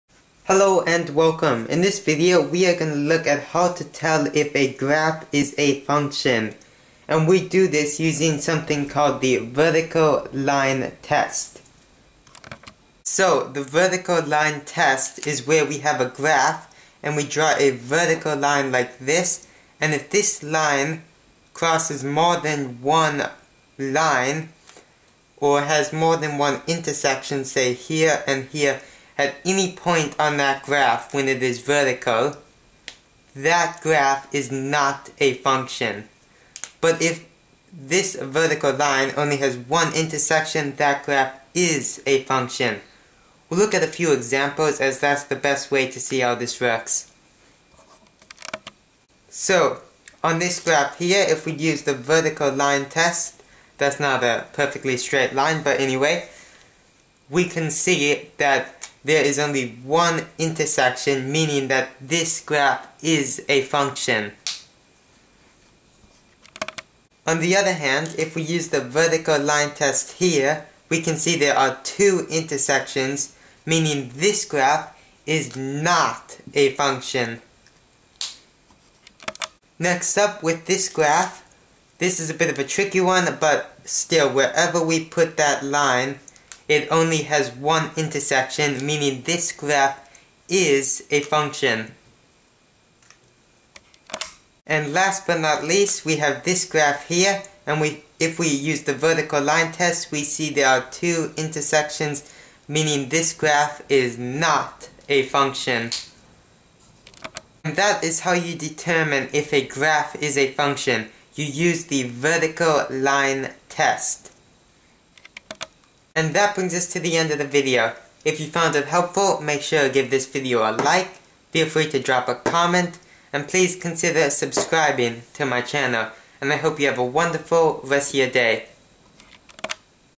How to tell if a graph is a function or not
Need to know how to tell if a graph is a function or not? Well you're in the right place! In this video we look at the vertical line test which helps us determine if a graph is a function or a relation. Being able to tell wether a graph is a function is relatively simple, but often forgotten. So I hope this video on how to tell if a graph is a function or not helps you in one way or another!